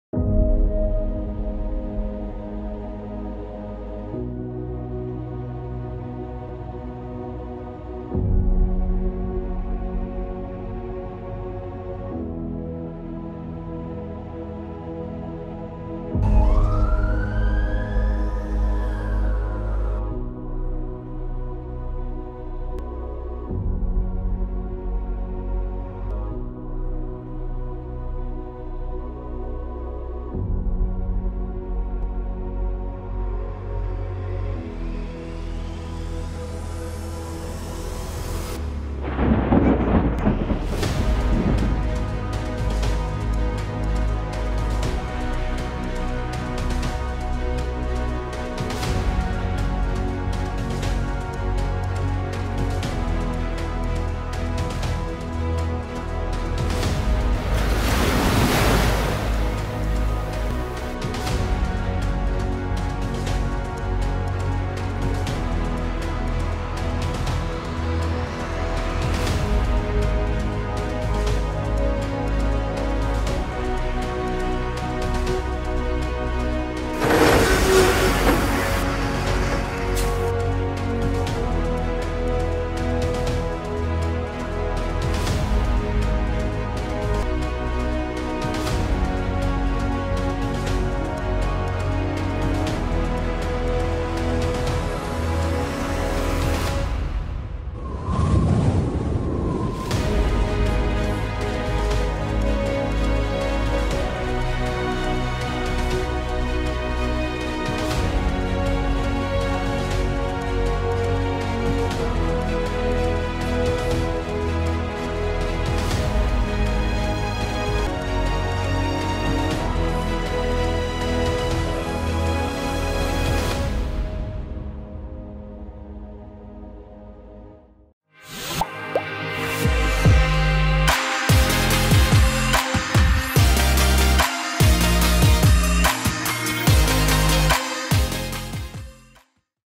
HWIDI-Huawei Intelligent Data Infrastructure for the Digital World
Today, data is shaping our digital world. Huawei Intelligent Data Infrastructure makes it easier, better, and more affordable for you to transform to digital. Discover more